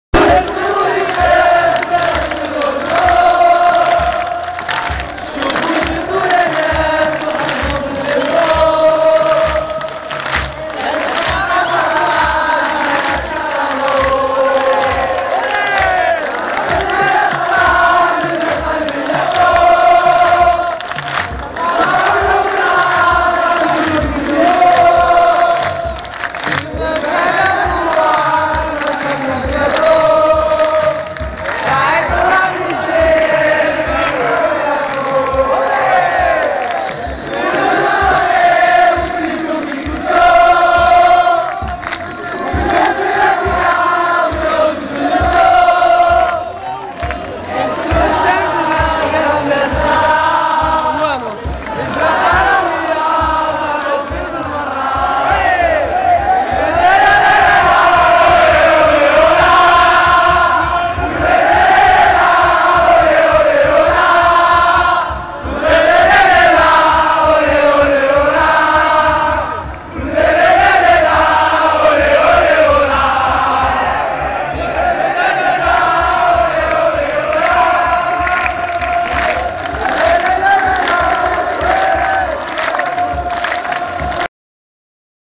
wydad faysali
suportere du wac lyoum jina ki l 3ada nzahaw f l curva nord